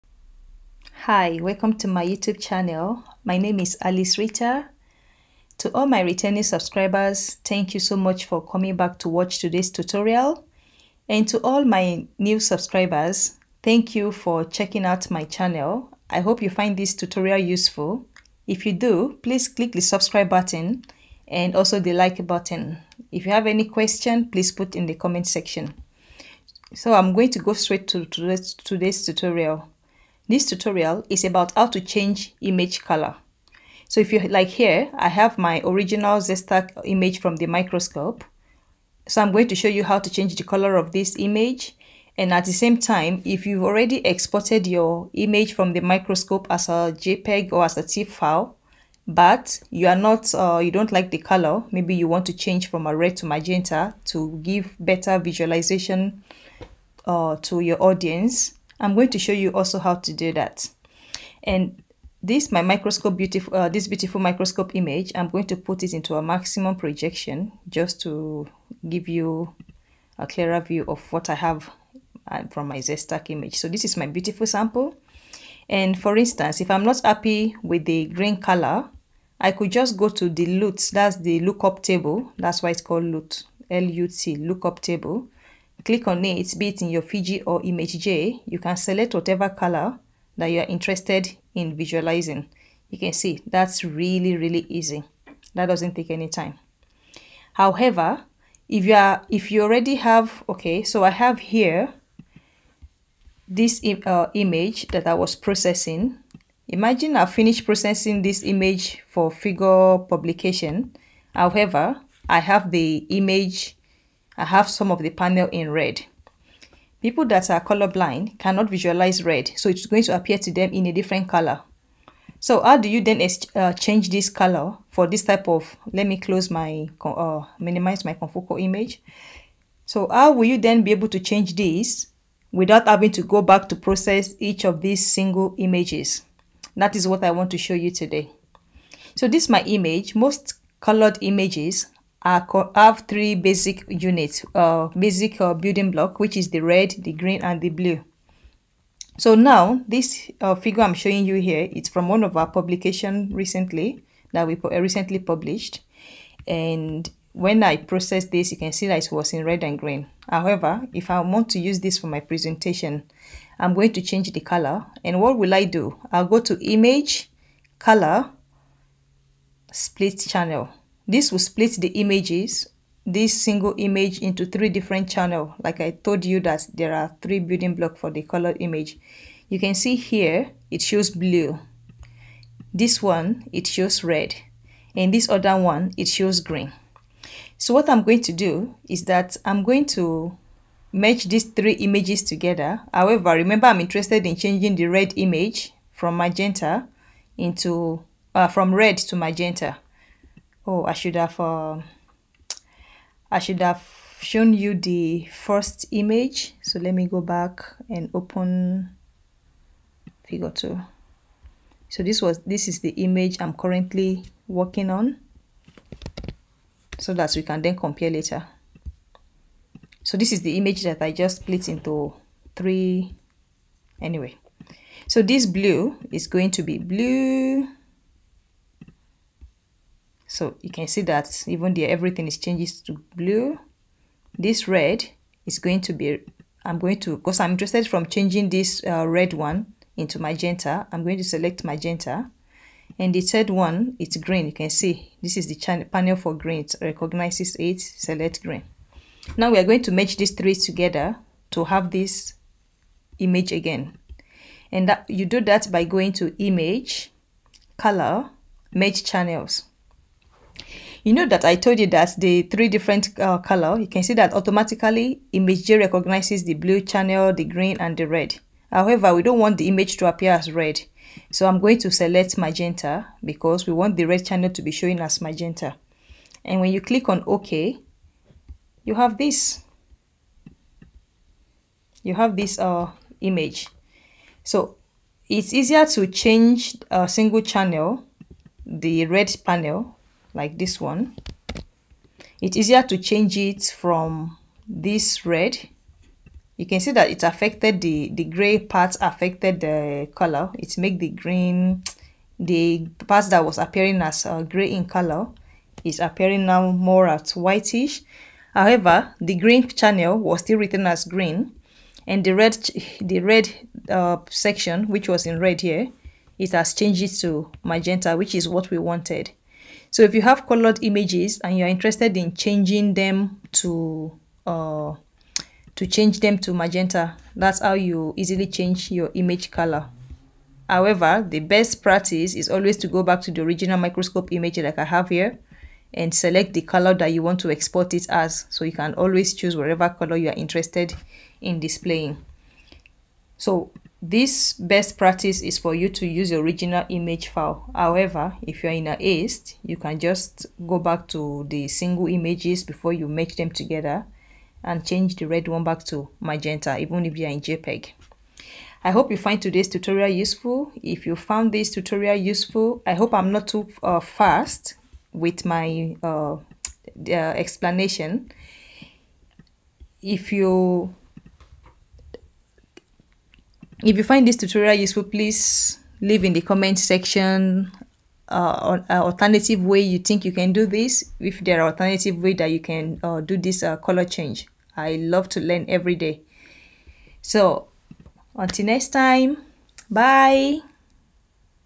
How to change microscope image colour in ImageJ| change video colour| how to pseudo colour in imageJ
Hi,
This video shows how to change microscope image colours. It's always a good practice to select colour-blind friendly options. The steps in this video are also applicable to changing the colour of the time-lapse microscope series.